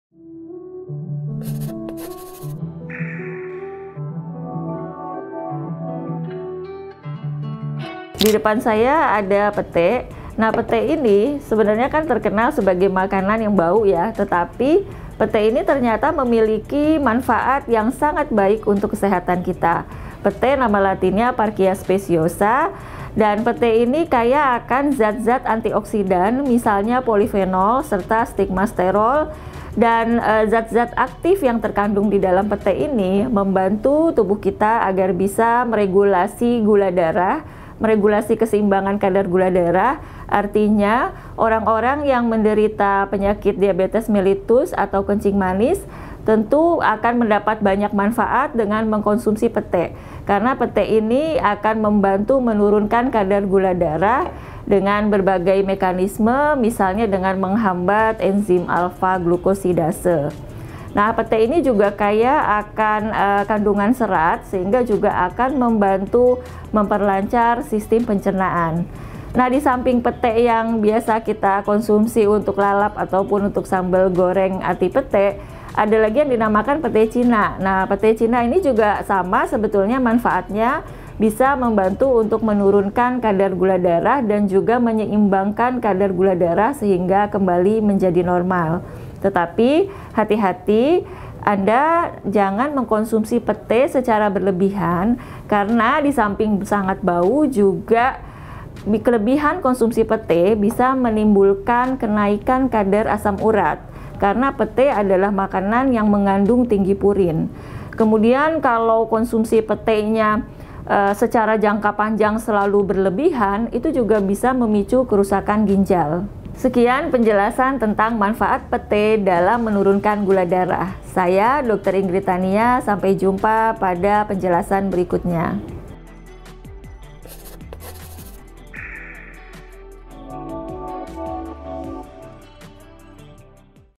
DASYAT! PETAI DAPAT TURUNKAN GULA DARAH | KATA DOKTER
Benarkah petai dapat menurunkan kadar gula darah dalam tubuh, dan baik buat penderita diabetes? simak penjelasan dokter inggrid tania hanya di kata dokter.